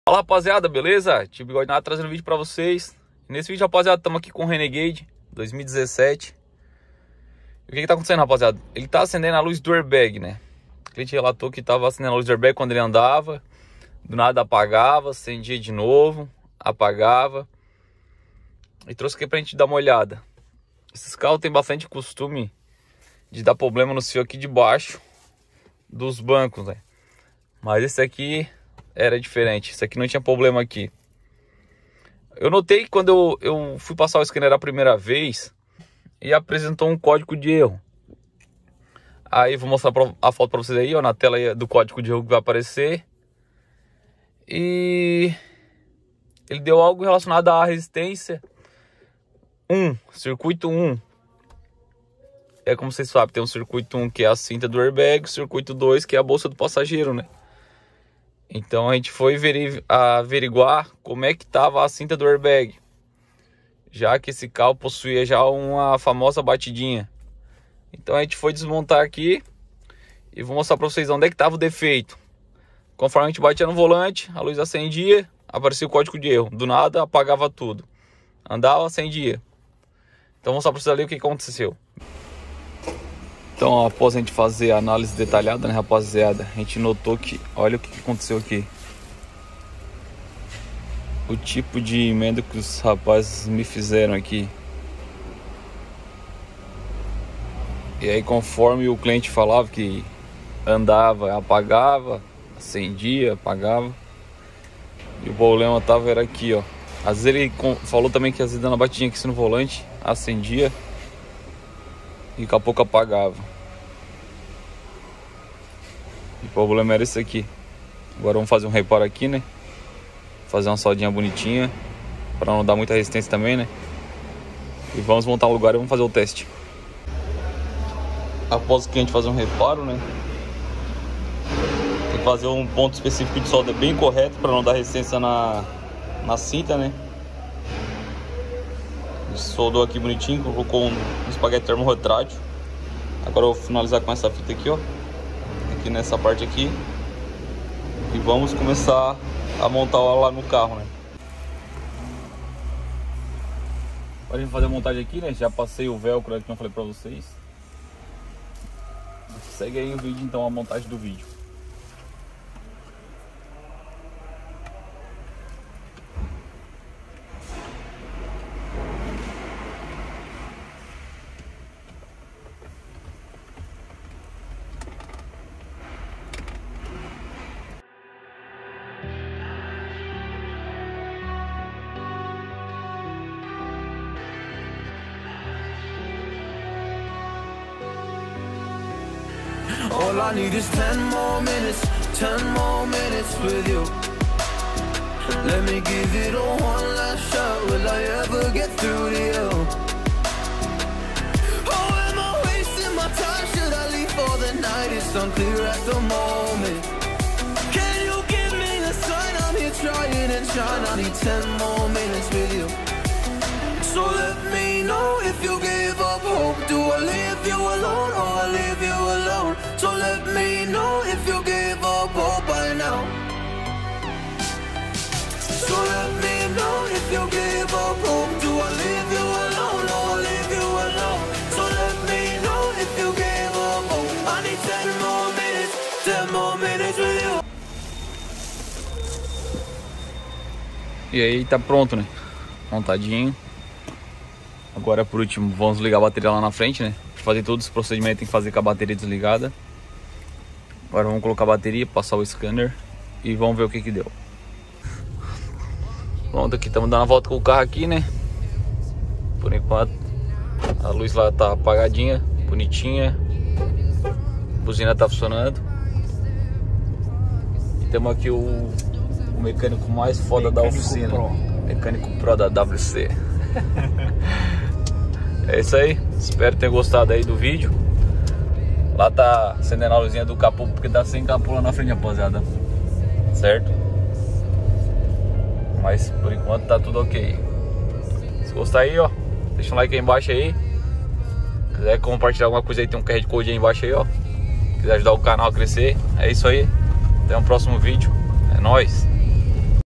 Luz airbag acessa Renegade Jeep luz airbag
Fala rapaziada luz de airbag acessa?

Ajude na vaquinha 🐮

scanner usado :KTS590